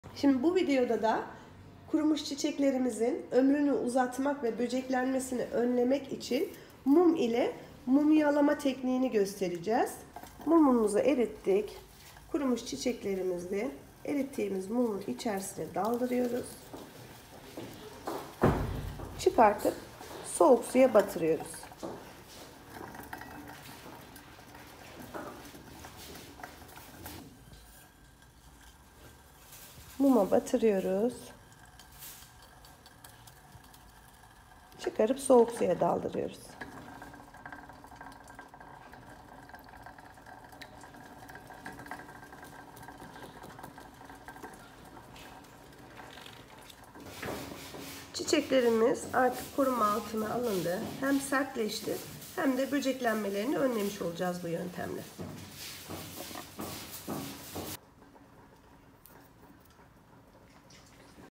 Kuru çiçek mumlama tekniği
Merhaba arkadaşlar. Kuru çiçekleri mum ile koruma altına alıp uzun ömürlü dökülmeden saklama yöntemi. Beğeni ve yorumlarınız çok kıymetli 💐🥰